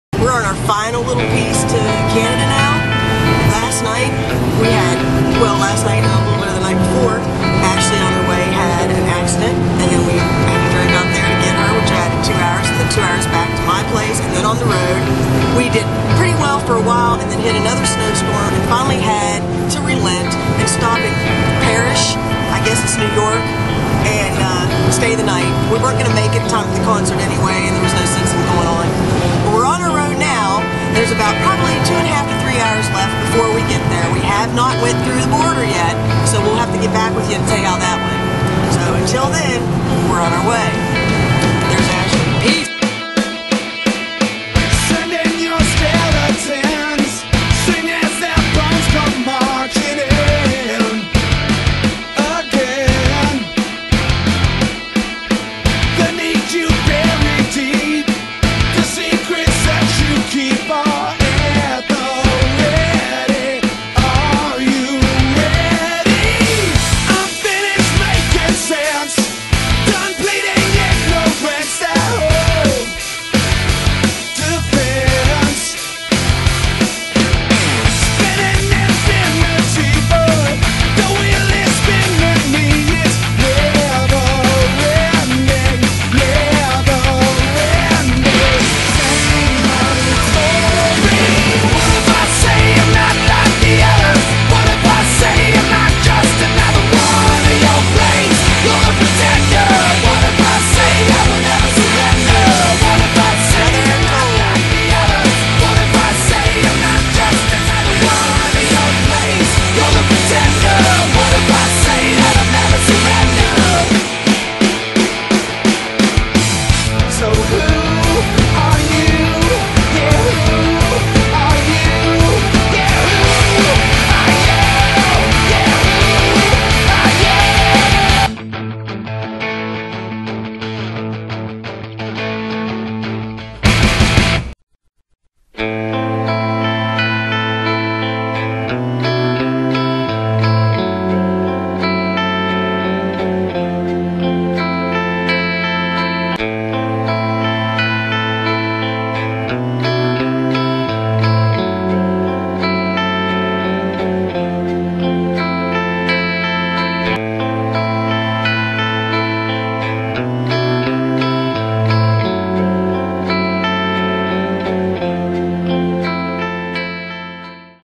To Canada or BUST - YFE road trip
This is part on in a series of videos from the long crazy weekend in Canada 2/28 - 3/2